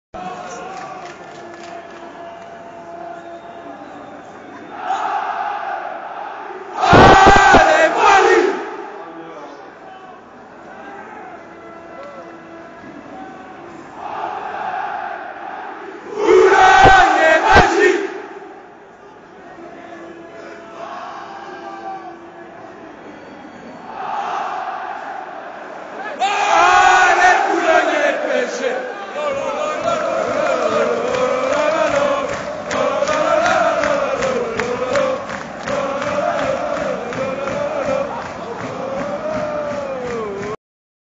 PSG Kayserispor
Ambiance début de match kop of boulogne

echange boulogne auteuil